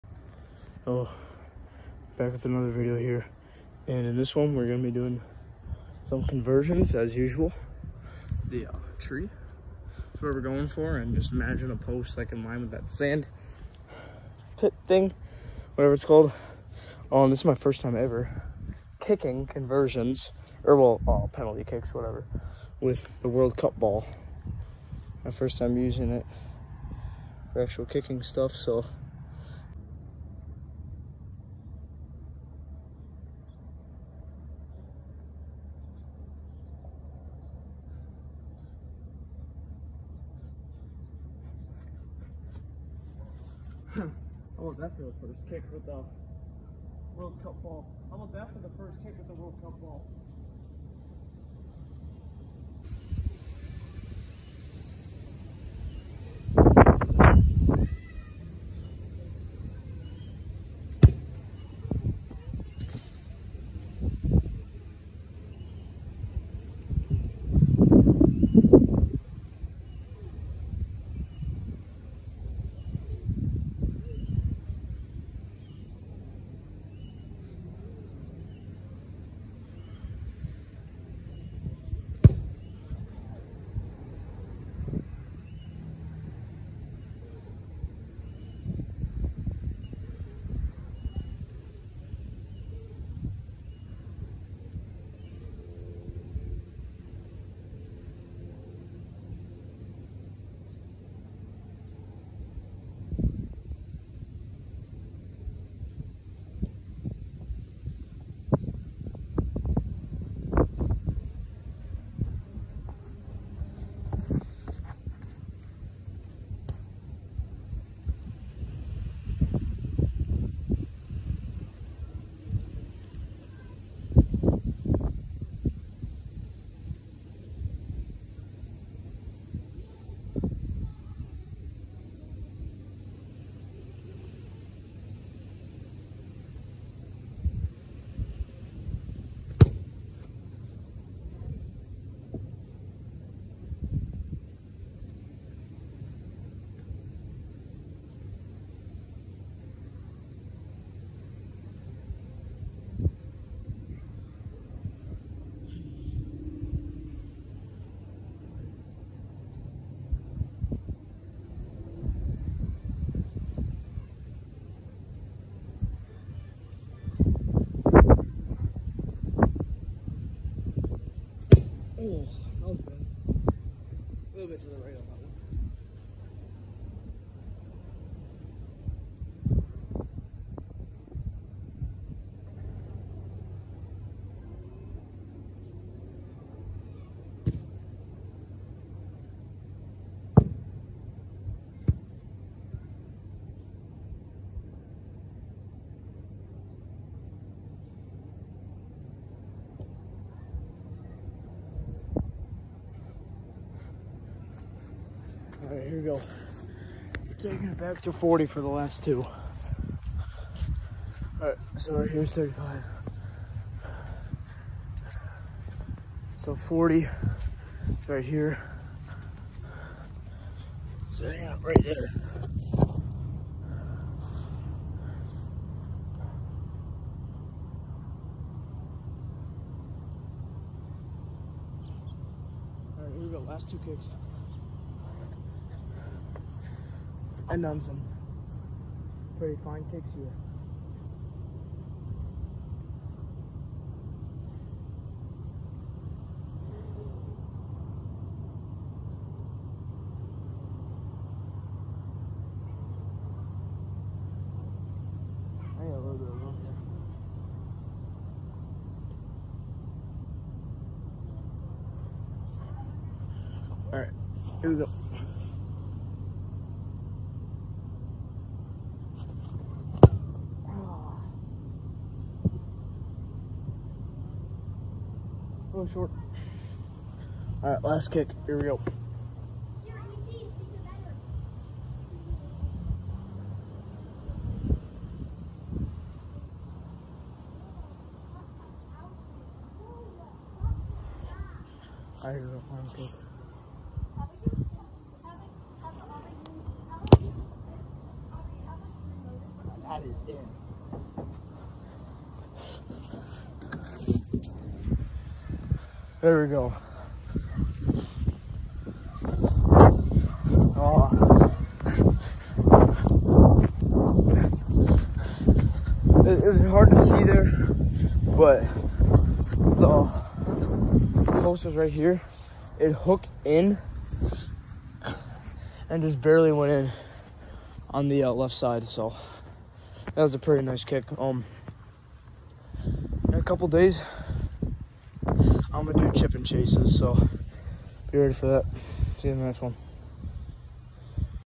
Penalty kicks with Rugby World Cup ball
I kick from 22m, 30m, 35m. I do well for the most part there last 2 kicks are from 40 meters.

(RWC 2023 ball, and original Gilbert are used)